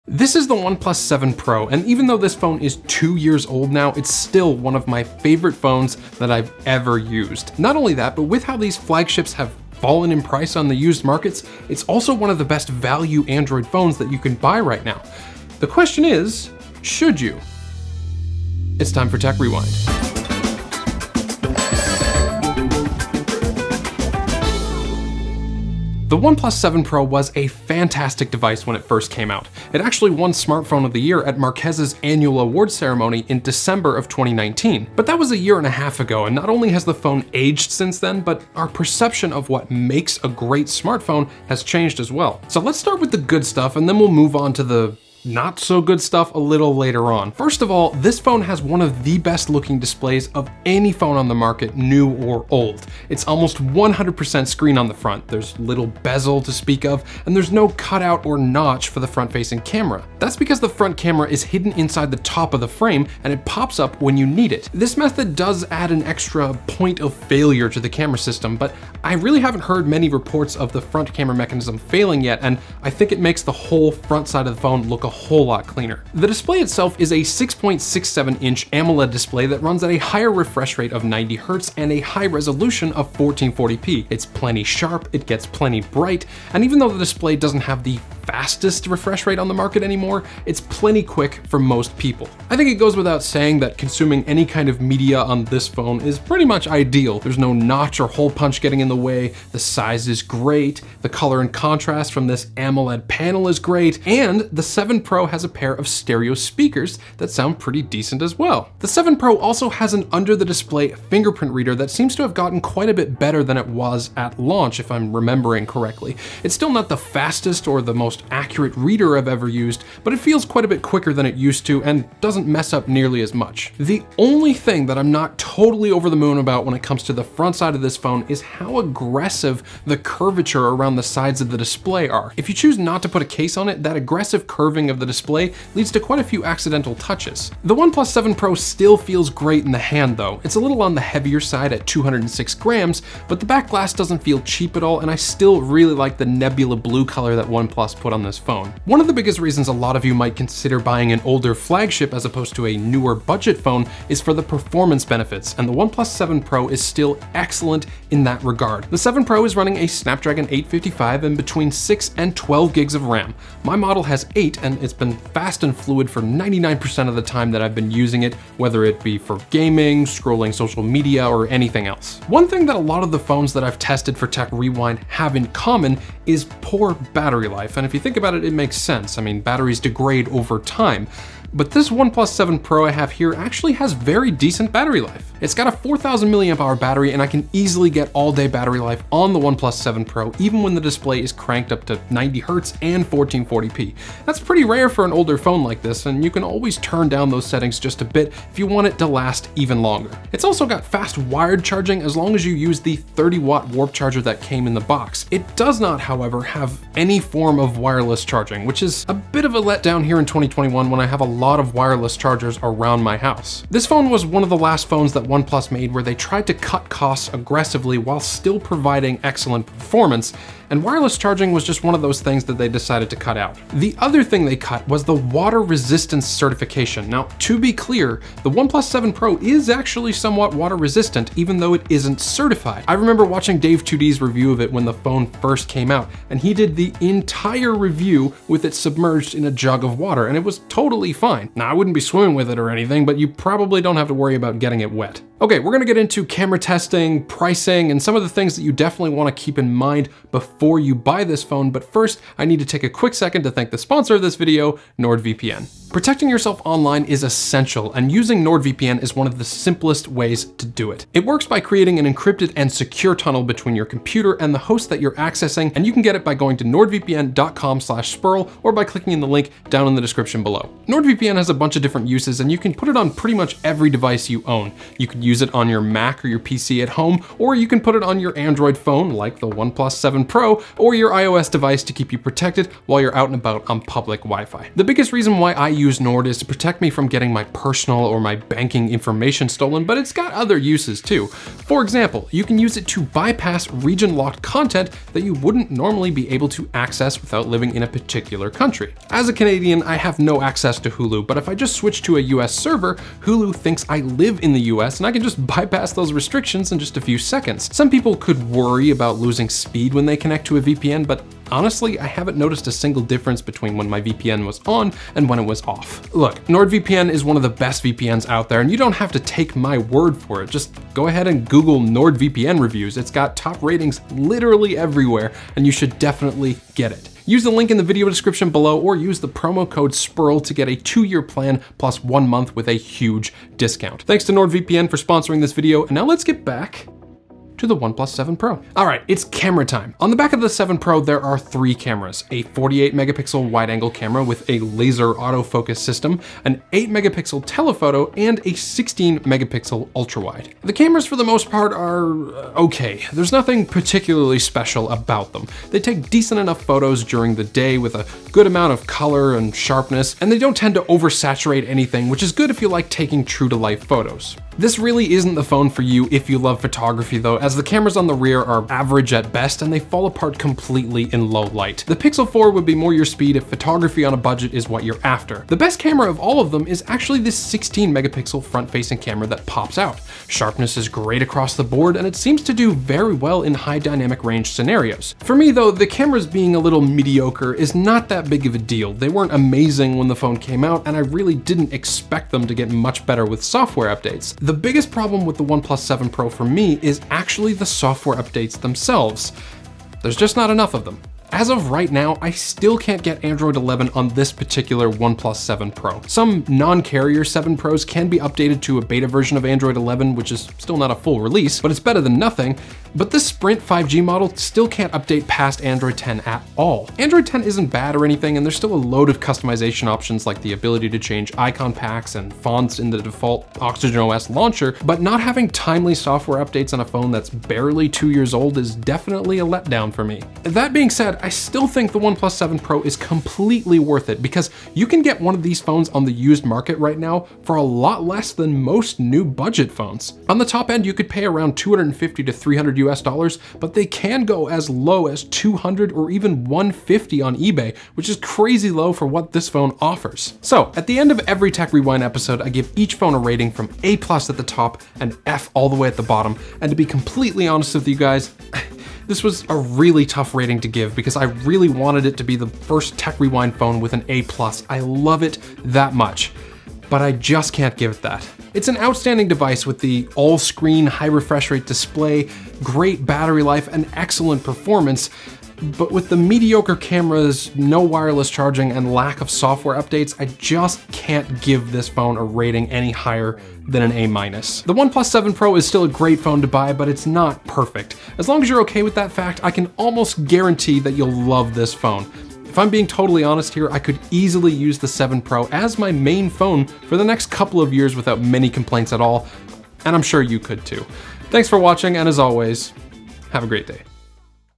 OnePlus 7 Pro: This Phone is Still INCREDIBLE!
The OnePlus 7 Pro was a huge hit among tech enthusiasts when it first came out, but how does it perform two years later? Let's find out on this episode of Tech Rewind!

My Gear:

Sony a7iii
Sony a6400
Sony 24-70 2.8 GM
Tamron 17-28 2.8
Edelkrone Slider Plus
Sachtler Ace M Tripod

Mark